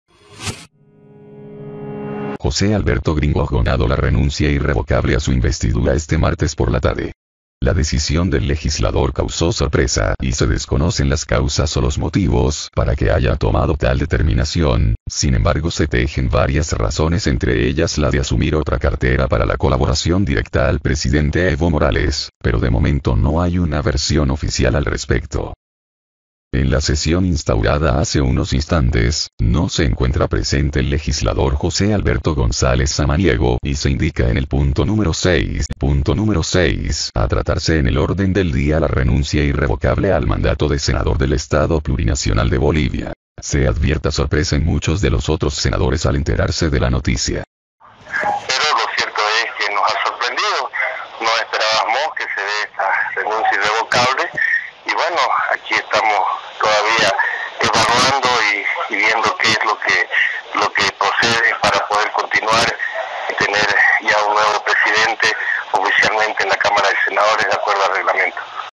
El presidente de la cámara de senadores "Gringo" Gonzales renuncia como senador
José Alberto 'Gringo' Gonzales presentó al Senado la renuncia irrevocable a su investidura este martes por la tarde. Las decisión del legislador causó sorpresa y se desconocen las causas o los motivos para que haya tomado tal determinación